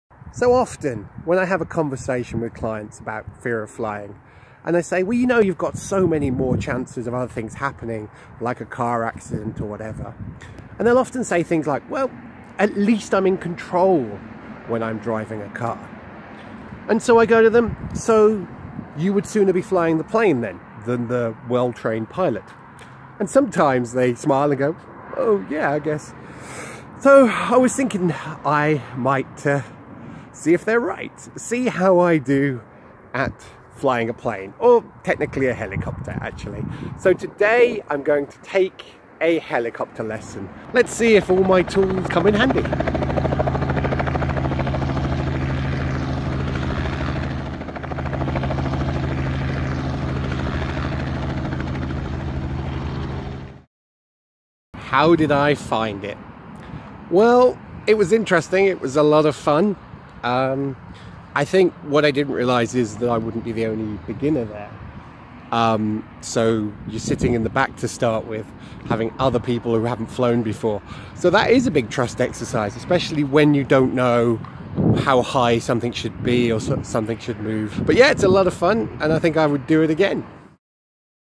Christopher's Flying Lesson
Christopher Paul Jones trys a Flying Lesson